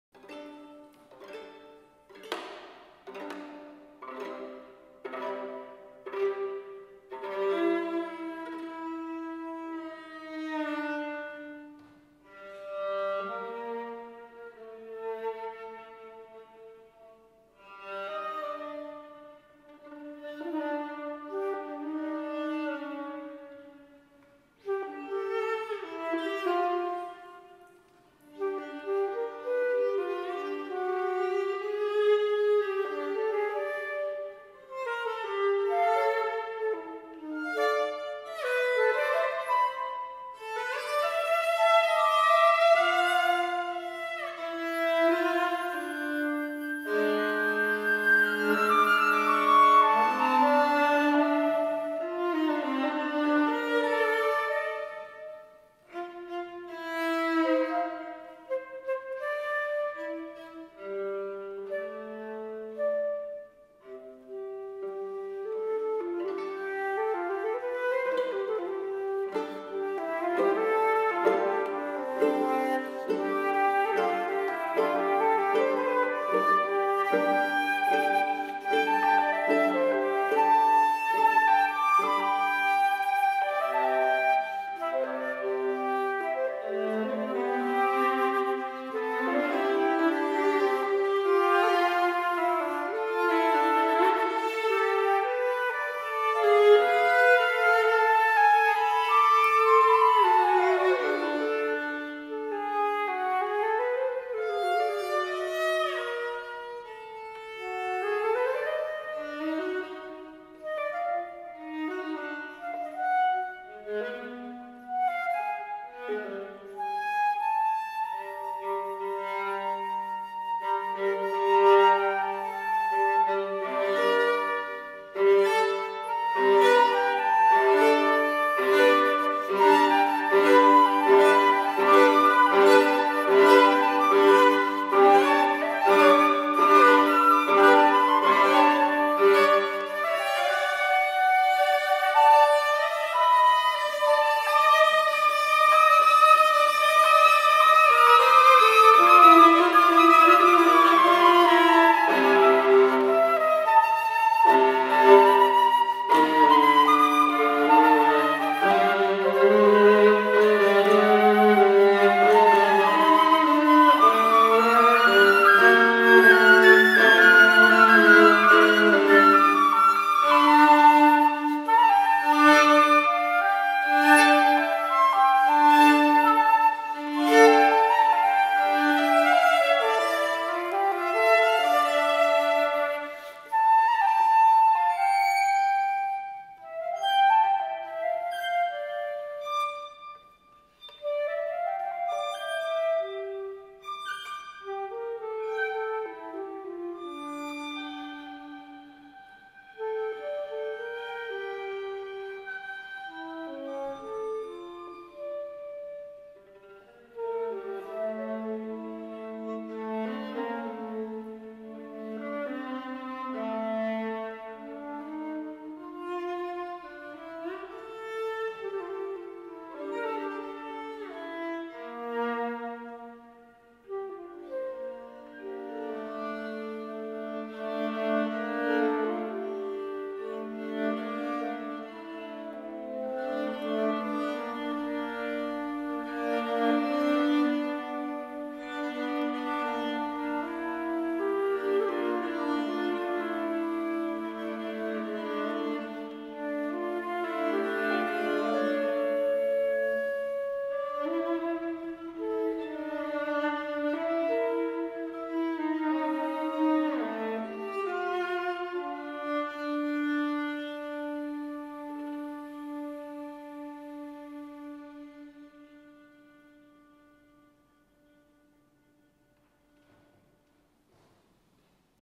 Music In Motion 43 - 23.11.2013
Tommaso Valenti - viola

improvvisazione estemporanea - free improvisation

registrato presso l' Oratorio degli Angeli Custodi - Lucca

audio registrato con uno zoom H2n

Immagine: Oratorio degli Angeli Custodi - Lucca